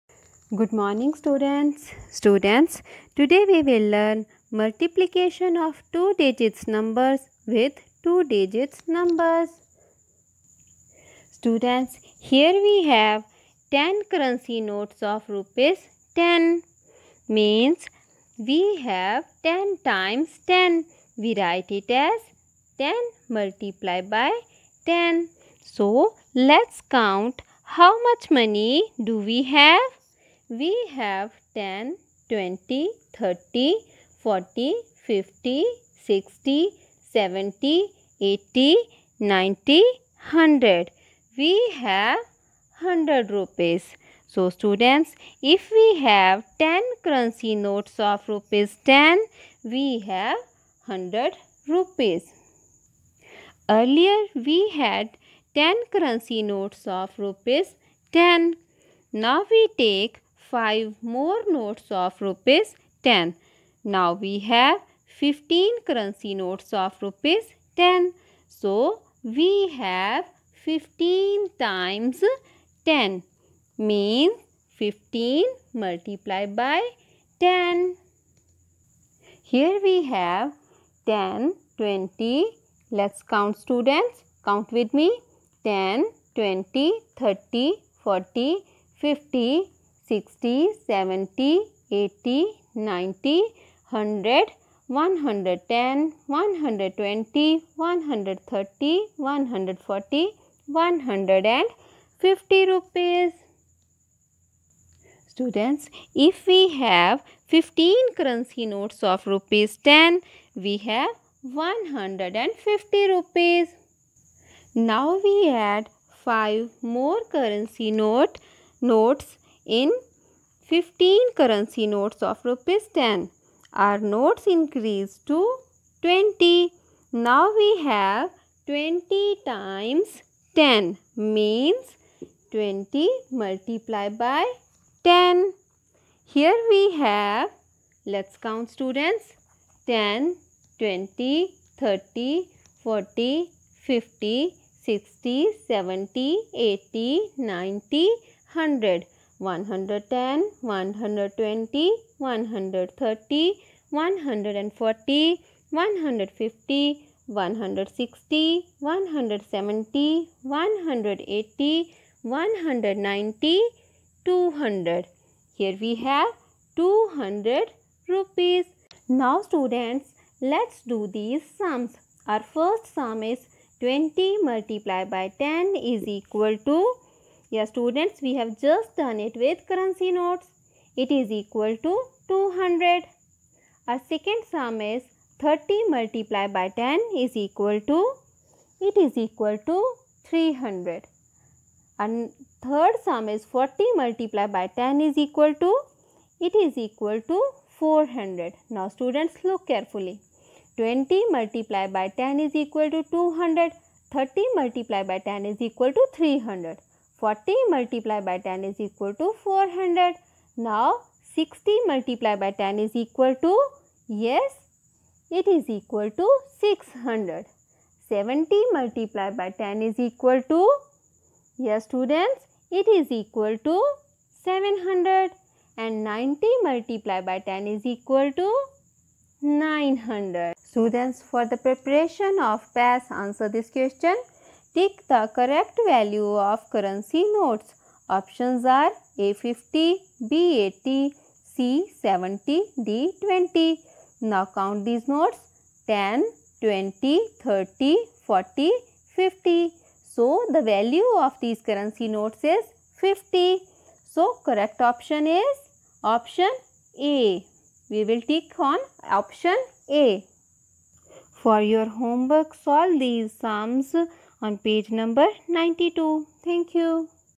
Learn multiplication of 2 digit number with 2 digit number in English
Maths world
Chapter : 3

Learn multiplication of  2 digit number with 2 digit number.